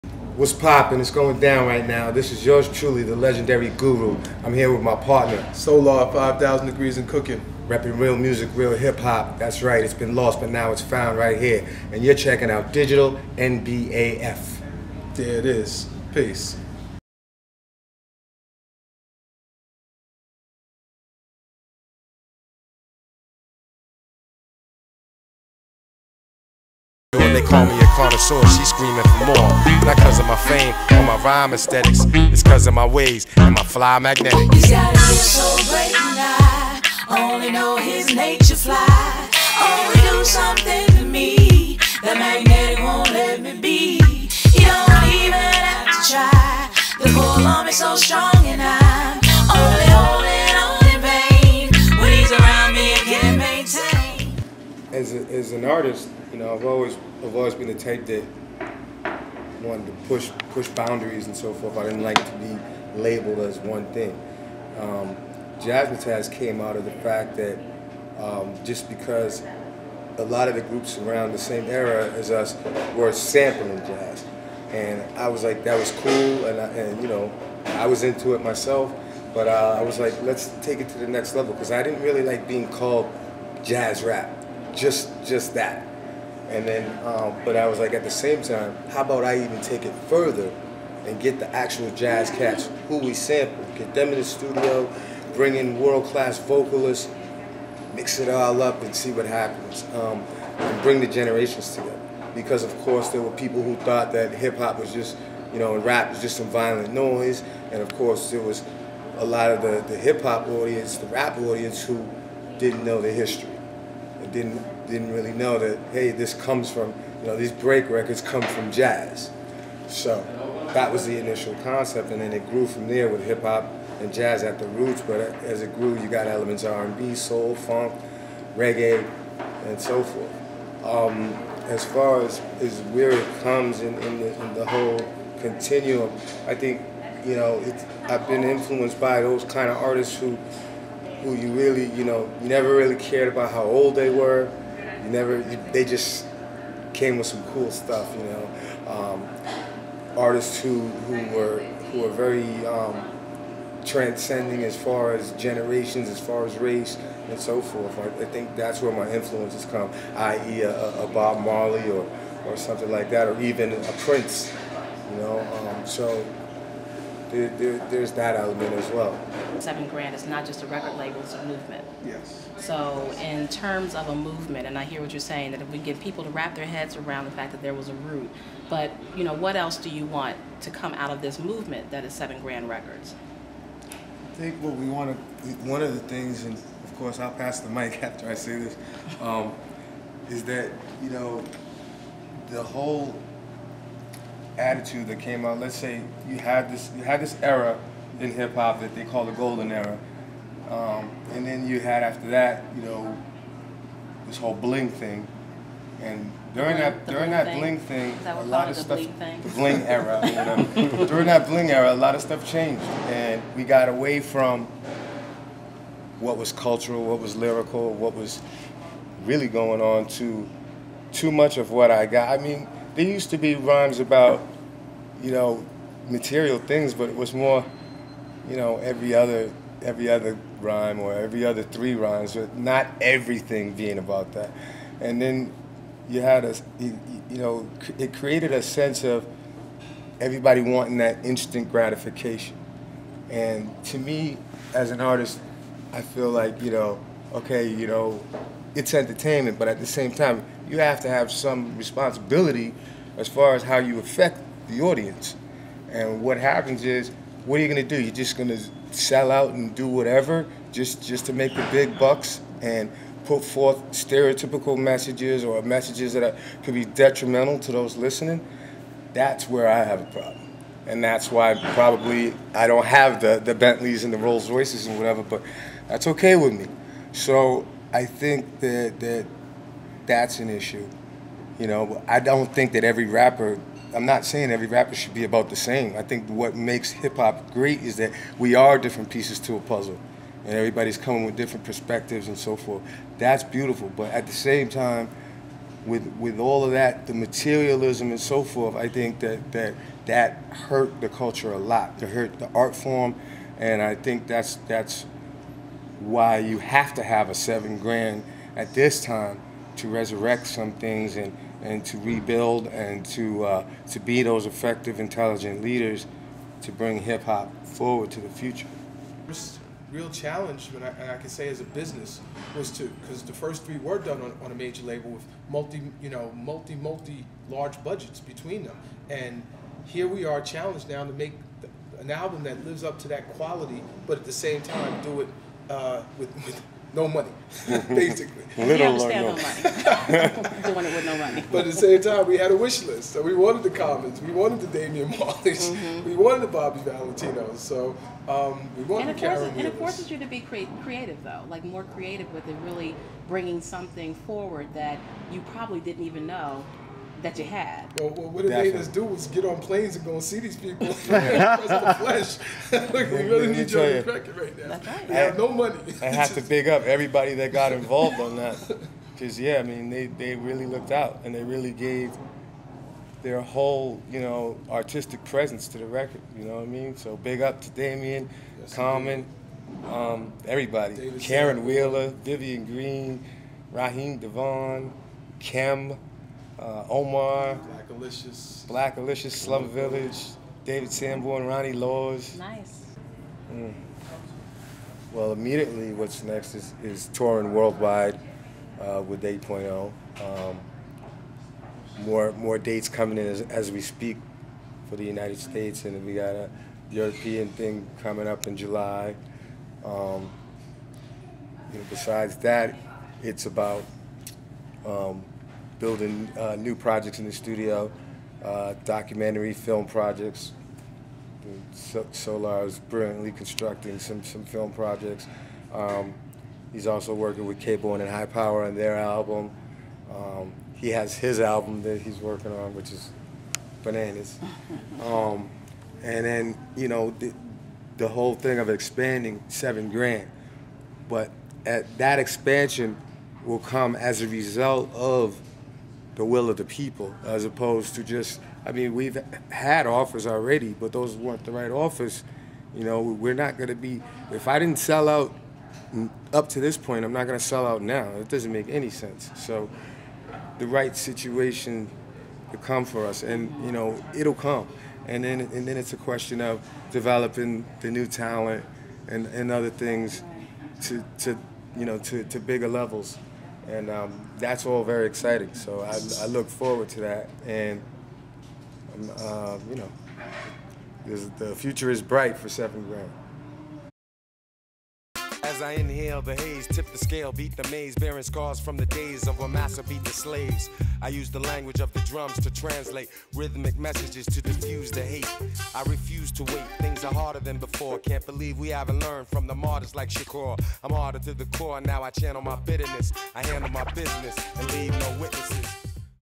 Interview with Guru
In May of 2009, Leatrice Ellzy sat down to talk with Guru about his life, career and future. The interview was initially going to be an audio podcast, but was also captured on a small handheld video camera for archival purposes. This is an edit of some of that footage.

Produced by
Leatrice Ellzy
Michael Moss

Edited by Malika Whitley

Interview Coordinator
Margaret Kargbo